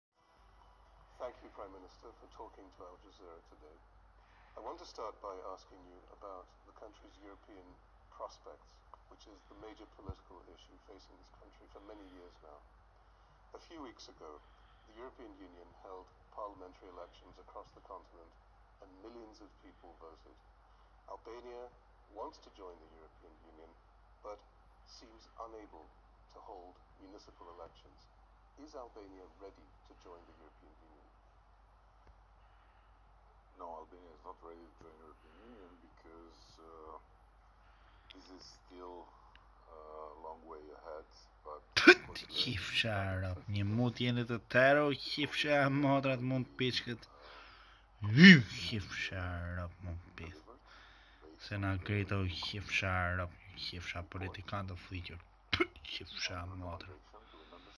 Edi Rama intervista AJ 17 ,06 ,2019
Al Jazeera pyet edi ramen nese shqiperia esht gati per join EU 🇪🇺 apo jo? Dhe ky kari thot jo nje mut qenkerkan te gjithe kta.